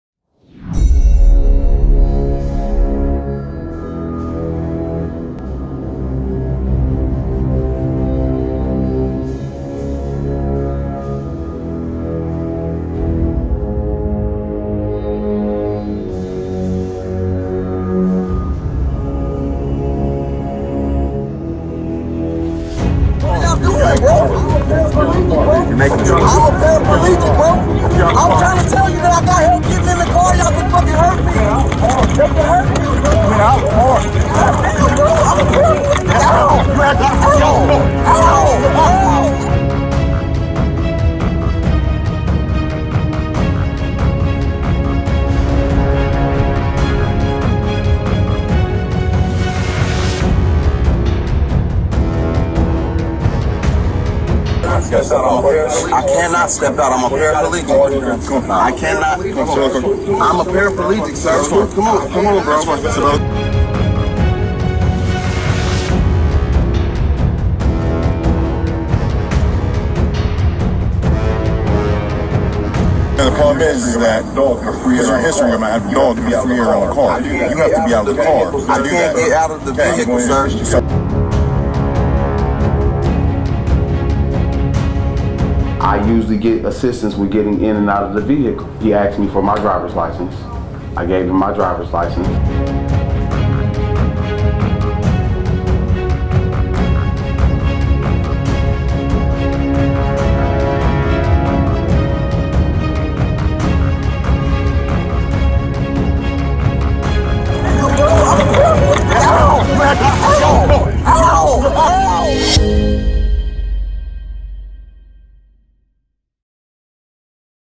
A disabled man was dragged by the police in Dayton, Ohio
Police in Dayton, Ohio dragged a paralyzed man from his car by his hair, a body camera footage shows.